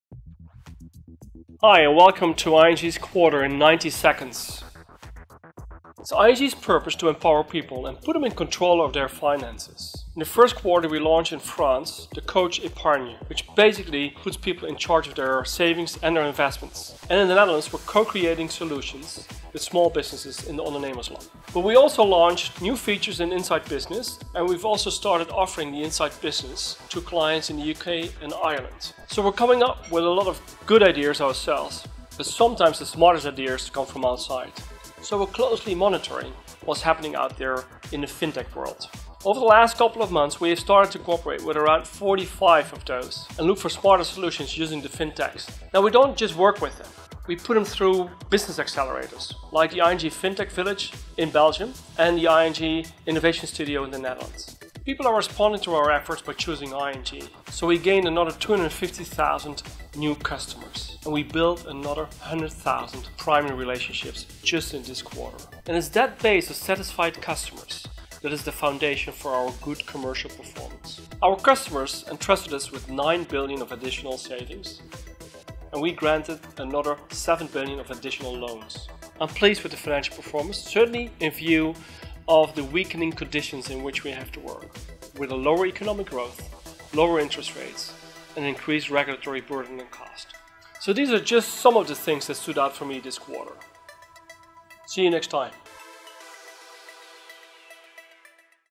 ING's 1Q16 results in 90 seconds (9 subtitles available)
ING CEO Ralph Hamers shares the highlights of our last quarter in 90 seconds.